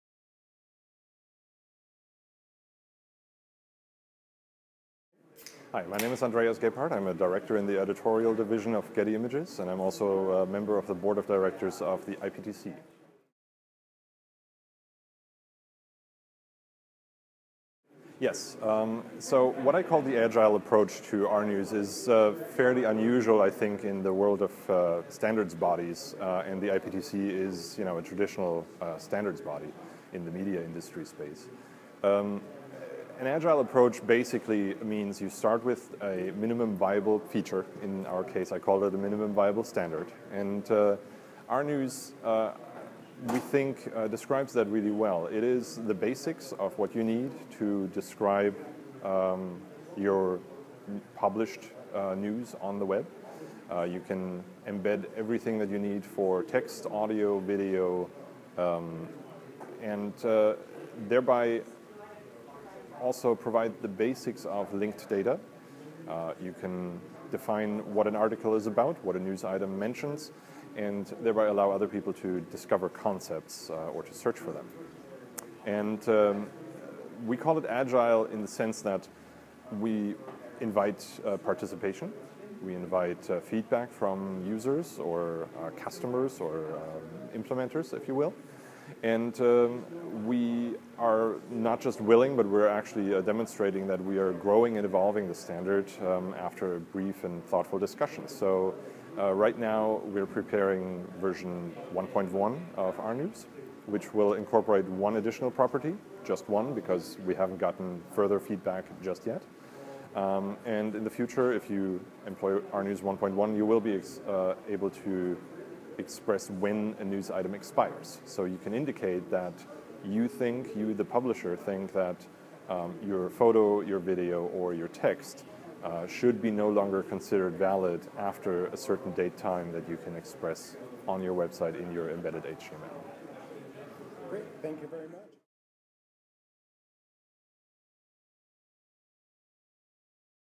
An Agile Approach To Standards Development
Andreas Gebhard (Getty Images and IPTC), discusses how the approach to the creation of rNews followed an agile methodology.